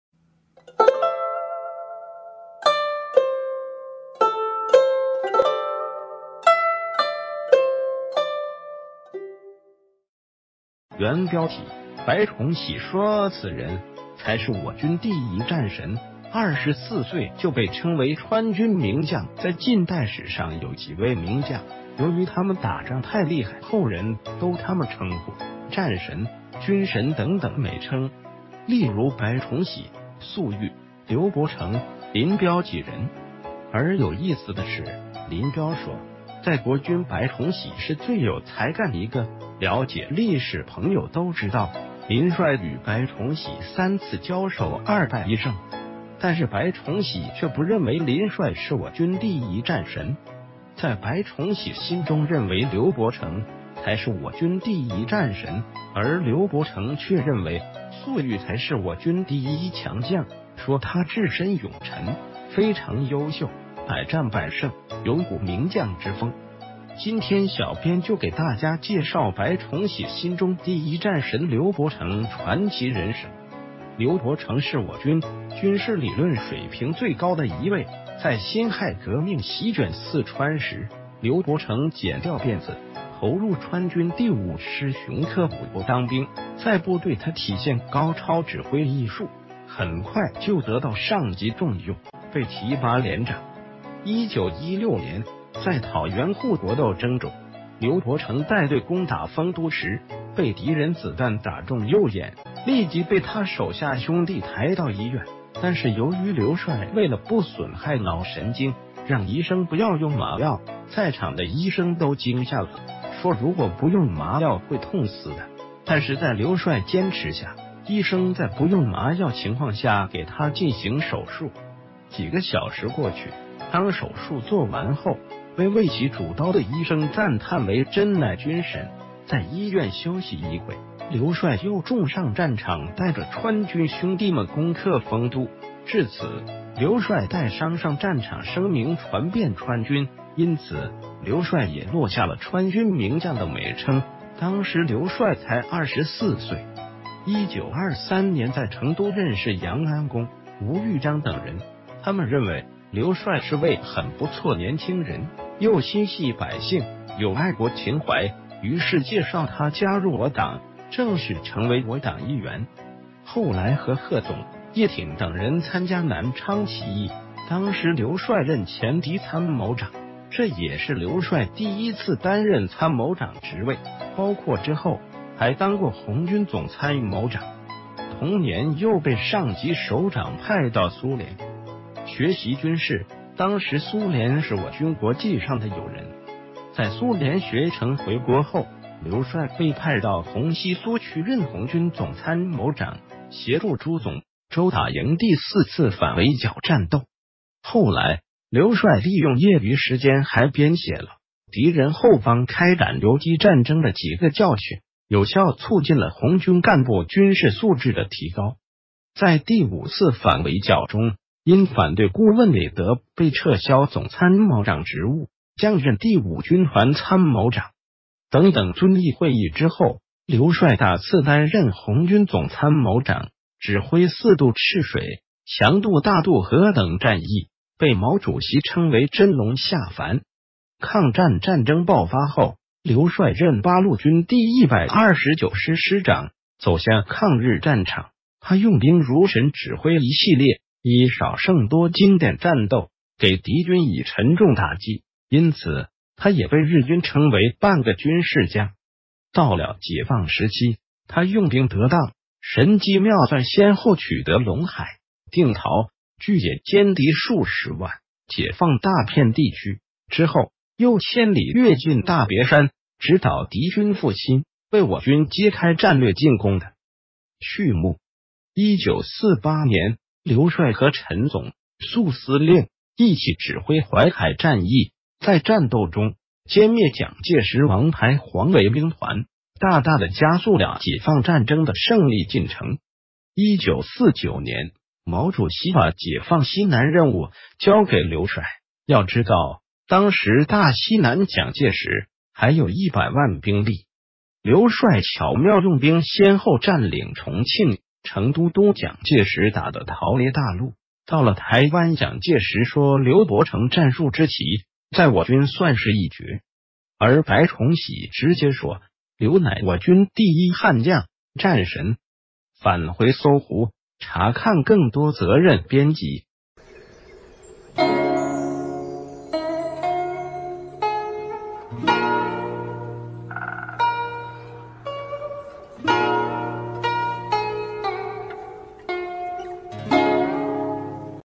白崇禧說此人，才是我軍第一戰神，24歲就被稱為川軍名將_劉帥
原標題：白崇禧說此人...